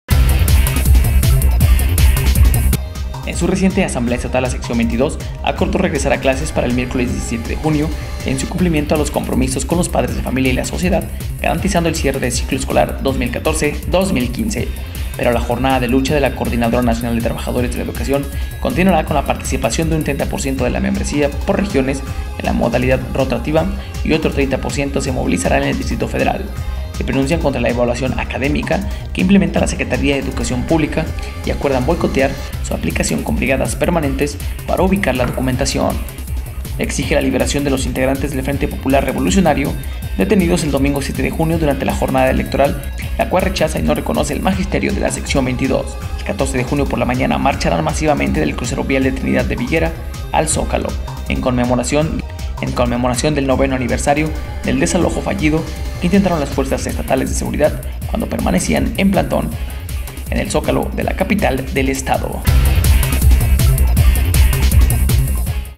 Sección 22 acuerda regreso a clases para el 17 de junio y continuar su lucha en forma representativa
En su reciente asamblea estatal la Sección 22, acordó regresar a clases para el miércoles 17 de junio en cumplimiento a los compromisos con los padres de familia y la sociedad; garantizando el cierre del ciclo escolar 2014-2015.